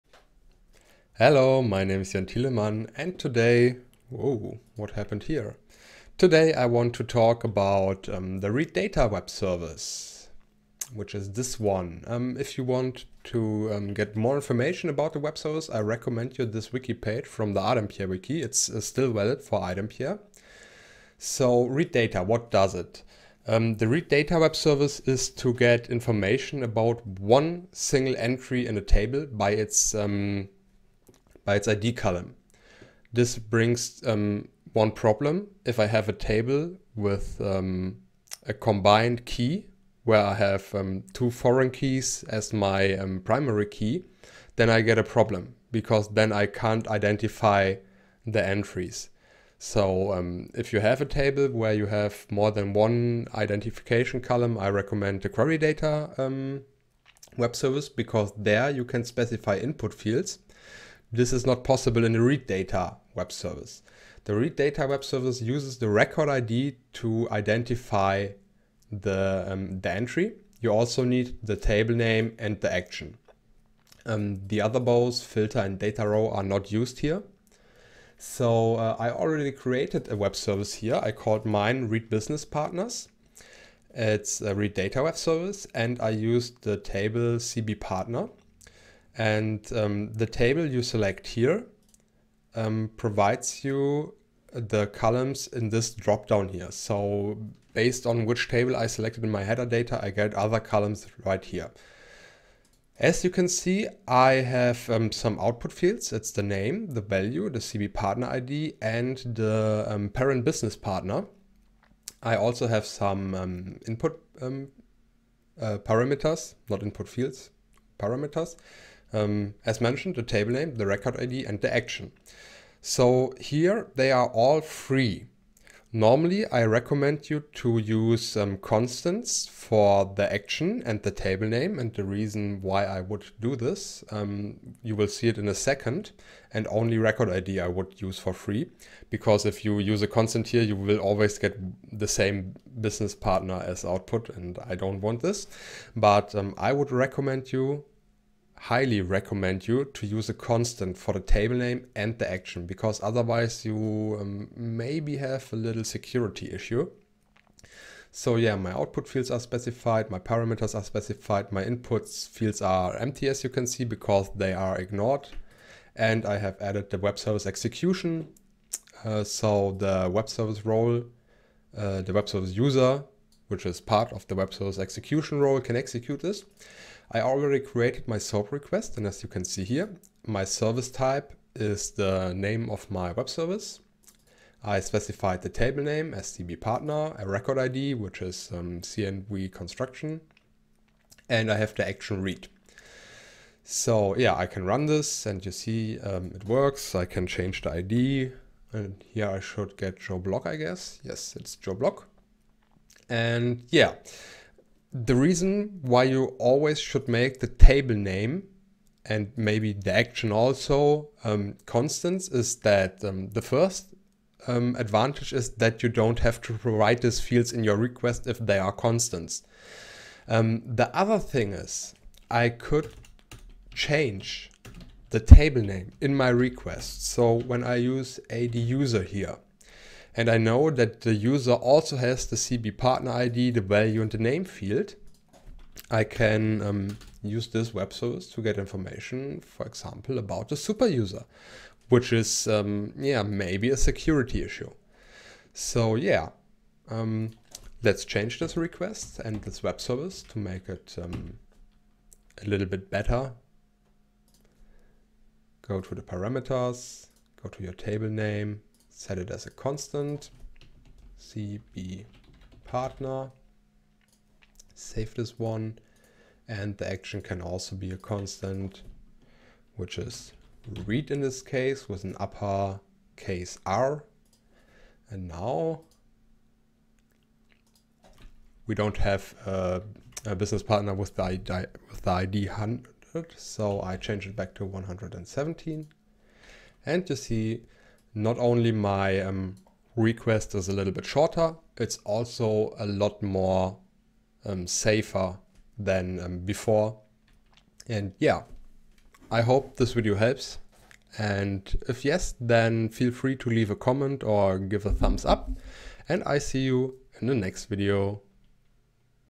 Developing iDempiere 2.0 - WebServiecs - readData
In this video you will learn how to use the readData Webservice from iDempiere